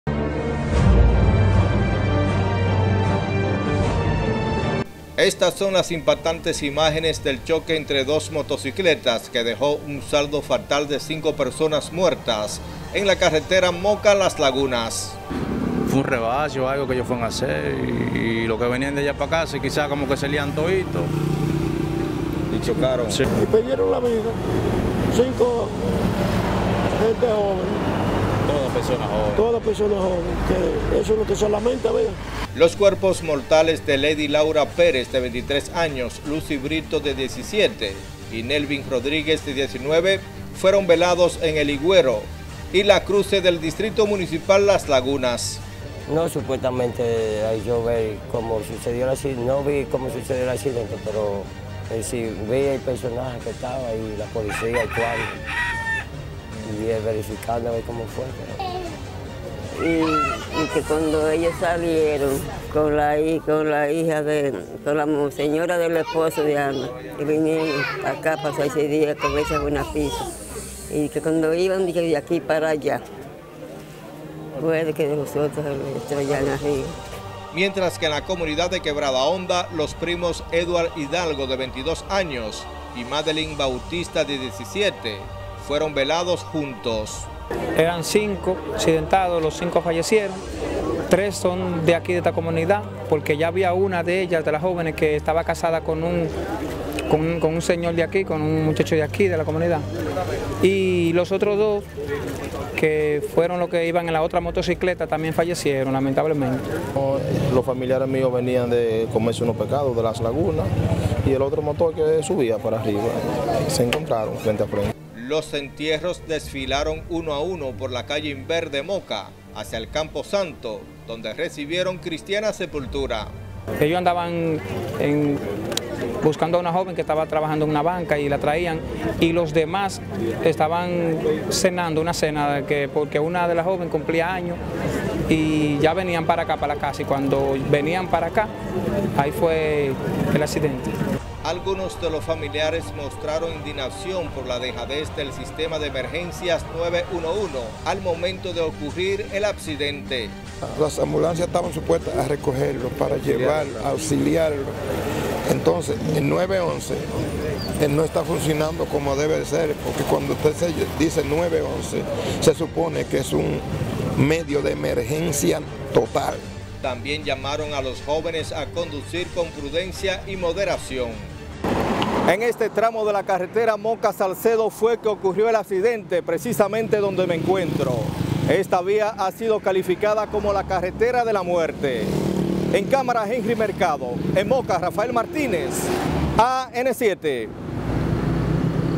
Choque entre motos deja saldo de cinco personas muertas en Moca
Moca/25/03/2018
Rafael Martínez/Duración  / 3: 03
Historia/ titulo: cinco muertos

En medio de dolor y consternación fueron sepultados los restos mortales de cinco jóvenes que murieron en un aparatoso choque de dos motocicletas en la carretera Moca-Salcedo. Rafael Martínez tiene todo los detalles.